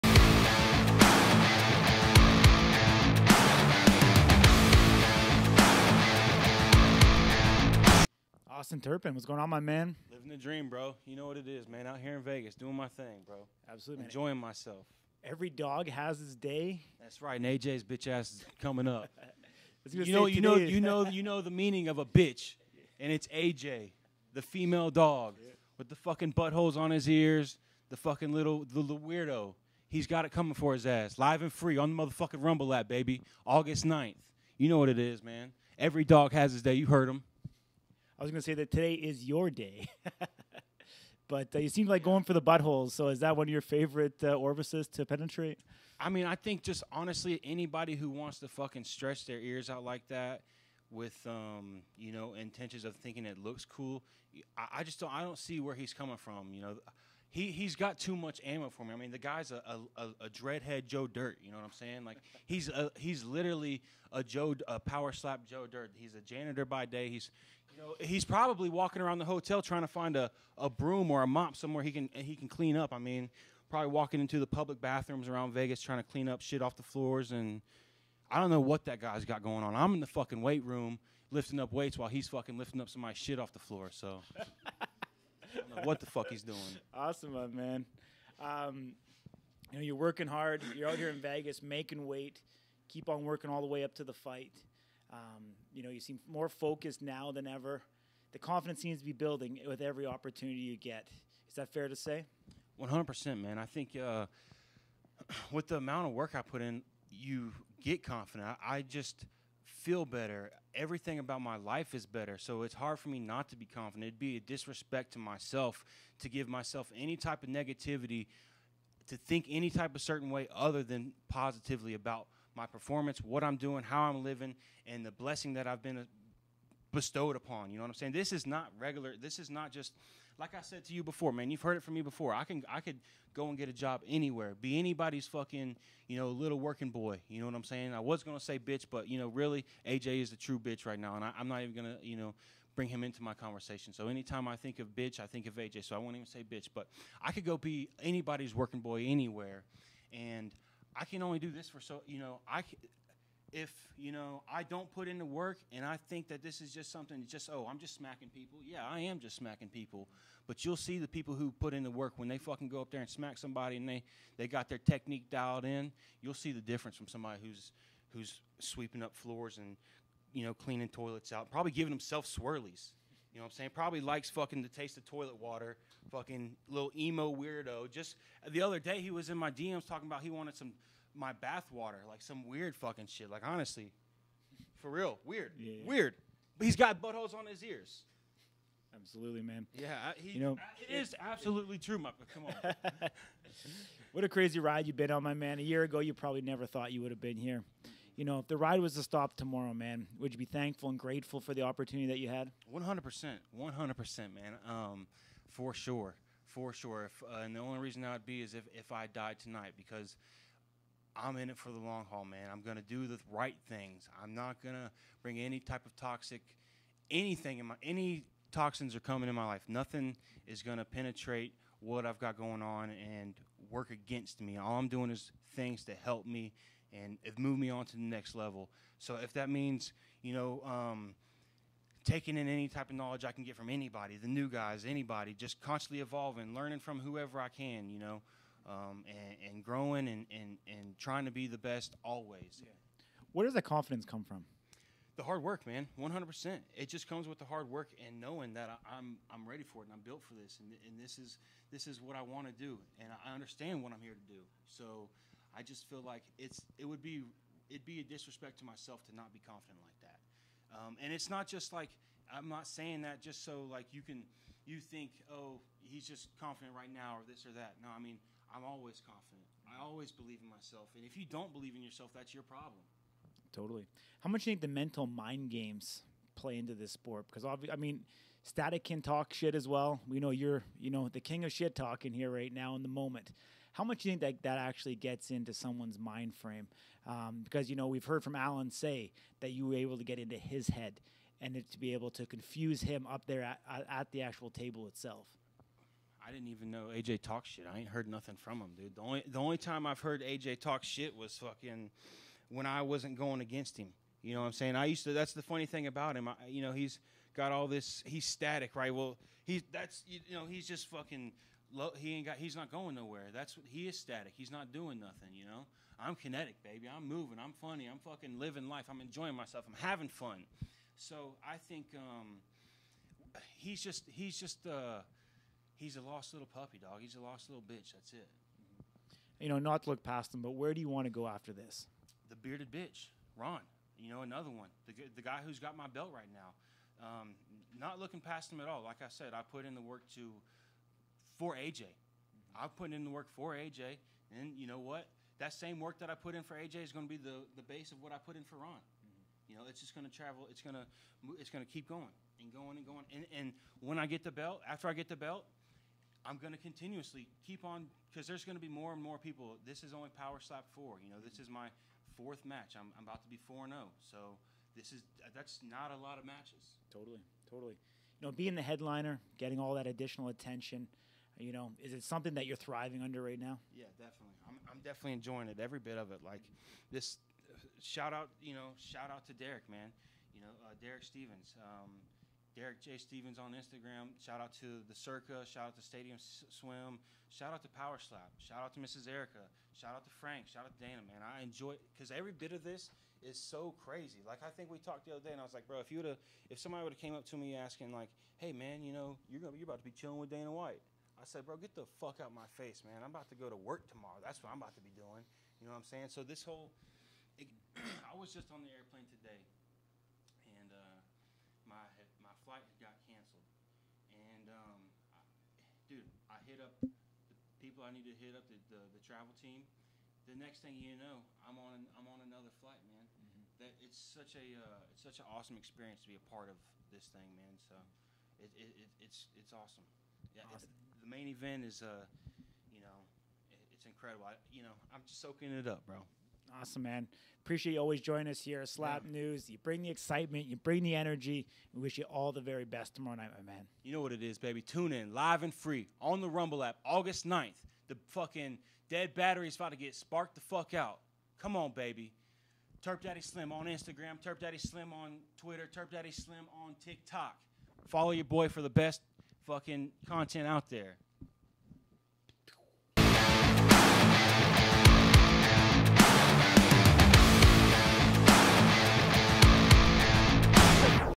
Power Slap 4 Behind the Scenes Interview Austin Turpin
Power Slap 4 Behind the Scenes Interview Austin Turpin. He gave us his thoughts on his PS4 opponent Ayjay "Static" Hintz, his lifestyle changes since becoming a striker, the mind games he plays on his opponent, and what he wants in his future for his career.